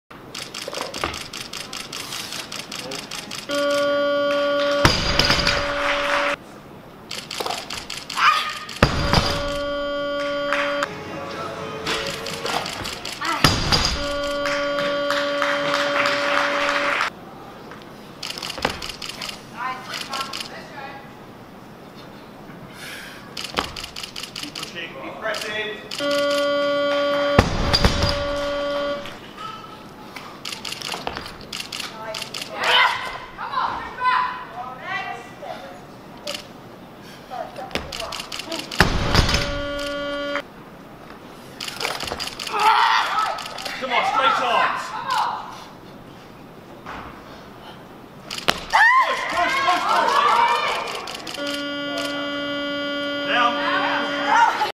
Lucy Barrett with a PB C&J of 70kg
Barrett Lucy (53kg) goes 48/70 for a PB total of 118kg at the English Championships. She's been chasing that 70kg C&J for a while now and as you can see she really fought to keep hold of it!